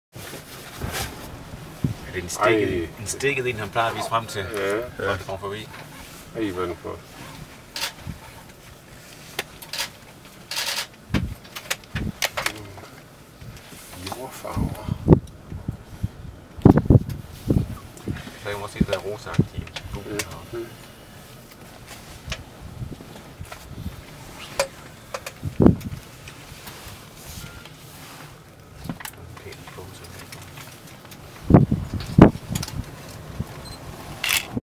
Sociable Plover (Steppevibe) Kasakstan 27/6 - 2010
Fra en fantastisk tur til Kasakstan med 11 andre fuglekiggere